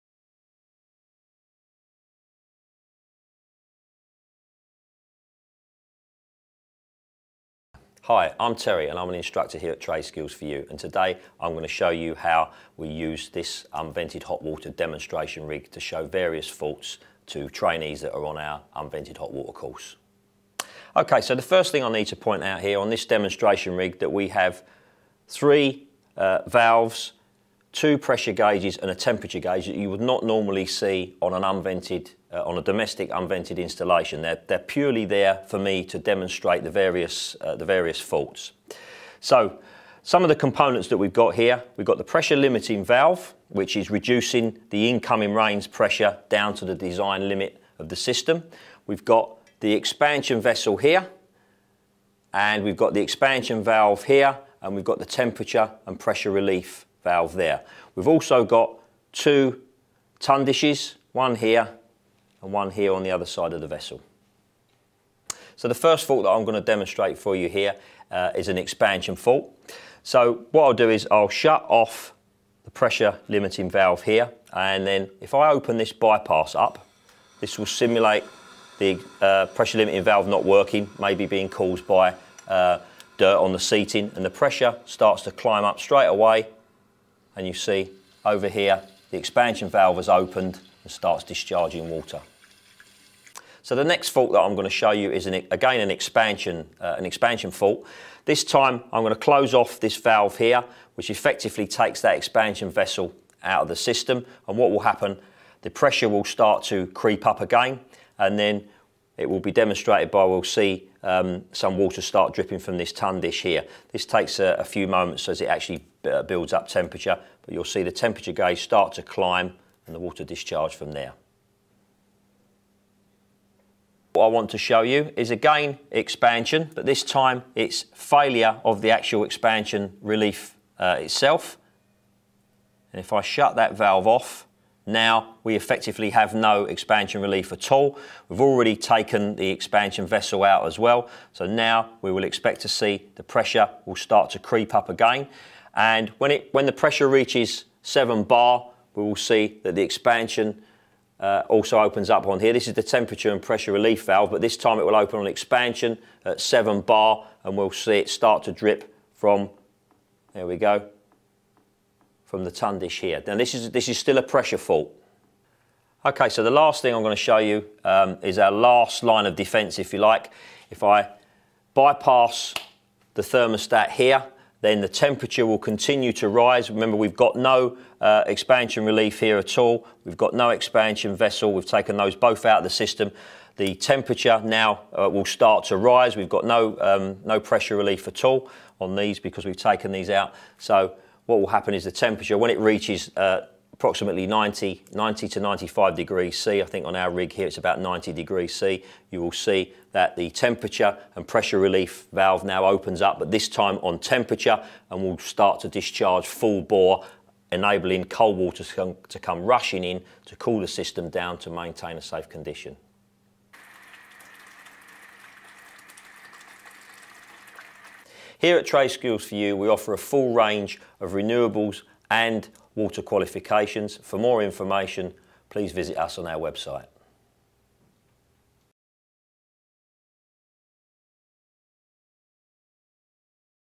How Unvented Hot Water Works
Showing you how faults develop in a unvented hot water system